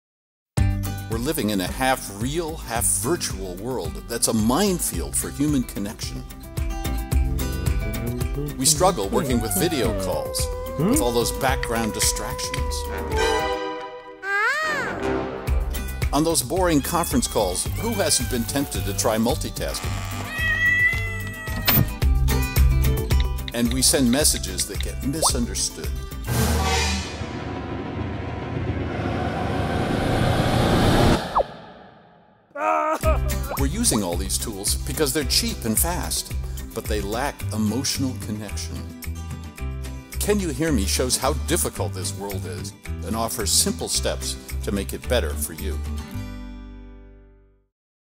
Can You Hear Me? Book Trailer
If you work or manage in an organization that has more than one office or customers who aren't nearby, Can You Hear Me? is your essential communications manual for the twenty-first-century.

How to Connect with People in a Virtual World
We’re immersed in the digital world; in fact, we couldn’t live without our mobile phones, computers, and social apps that allow us to work and play in ways unimaginable even two decades ago. But the blurring of the virtual and the real has a hidden cost that we pay every day. The technology strips ordinary human emotional understanding out of our communications, and we are the poorer for it – not to mention the misunderstandings, miscommunications, and mistakes that arise from emails that convey the wrong tone, texts that misuse humor, and audio conferences that put everyone to sleep. In Can You Hear Me?, Nick reveals what is missing and shows how to put some of the human connection back into our emotionally impoverished virtual world.